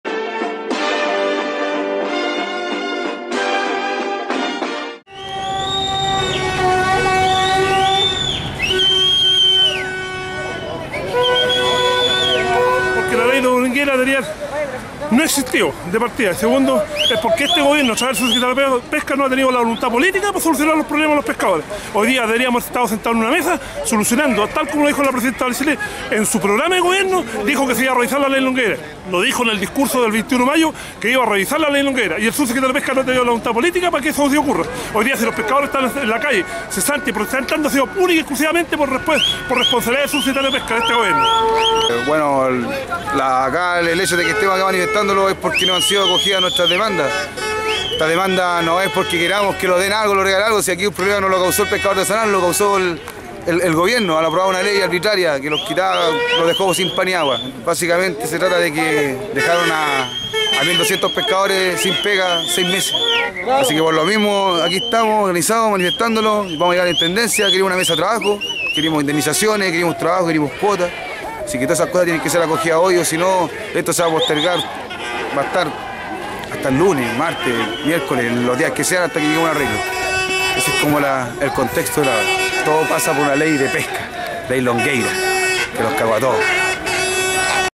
Pescadores marchan por el centro de Valdivia
Alrededor de un centenar de pescadores artesanales del bacalao y sus familias iniciaron un proceso de movilización durante este viernes 5 de Agosto. La reducción de las cuotas de captura ha cortado de cuajo su fuente laboral desde hace dos meses y la restricción se extenderá por 4 meses más. Los pescadores han increpado a la presidenta a que cumpla su promesa de revisar la Ley Longueira, la cual ha causado la última arremetida del modelo contra un precarizado sector artesanal que hoy lucha por subsistir.